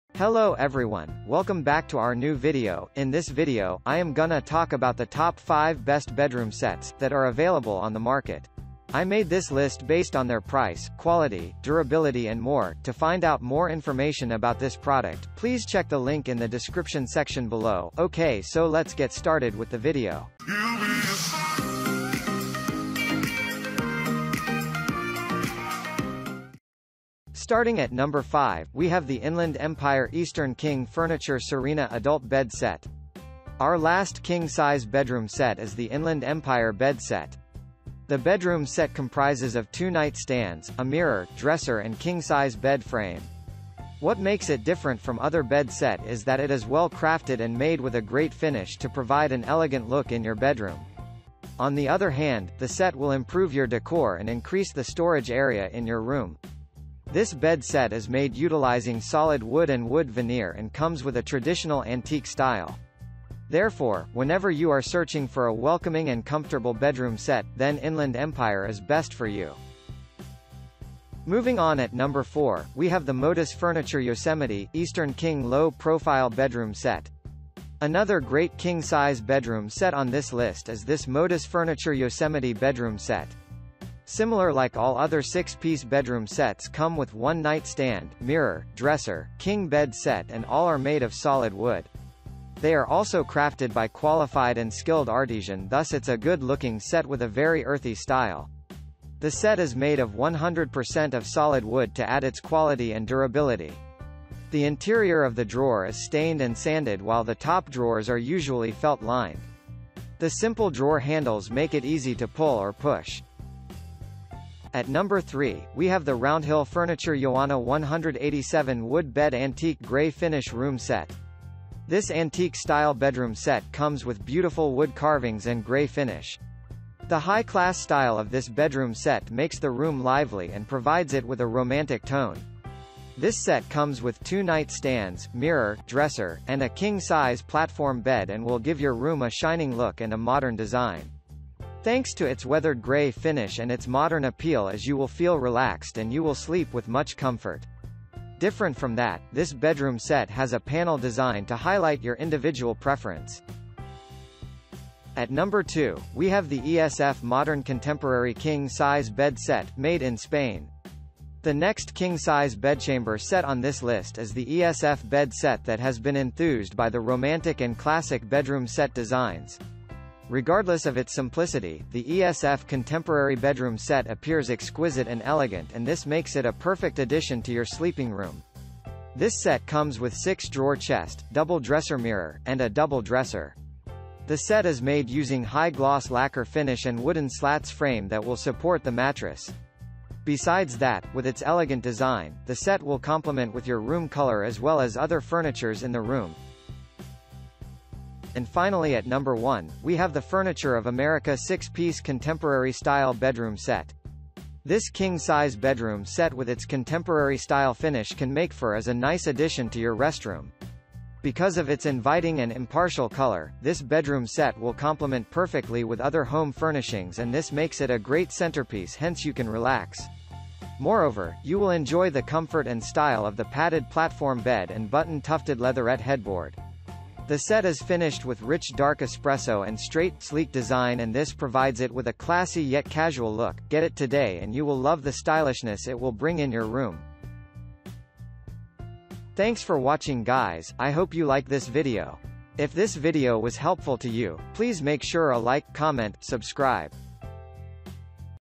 Top 5 Best Bedroom Sets in 2019
☛ All The Links to Best Bedroom Sets listed in this Video:-

► 5. Inland Empire Eastern King Furniture Serena Adult Bed Set

► 4. Modus Furniture Yosemite, Eastern King Low Profile Bedroom Set

► 3. Roundhill Furniture Ioana 187 Wood Bed Antique Grey Finish Room Set

► 2. ESF- Modern Contemporary King Size Bed Set, Made in Spain

► 1. Furniture of America- 6-Piece Contemporary Style Bedroom Set

In This Video, I listed the Best Bedroom Sets That are available in the market in 2019. I made this list based on my personal research, and i tried to list them based on their price, quality,  and Many more things.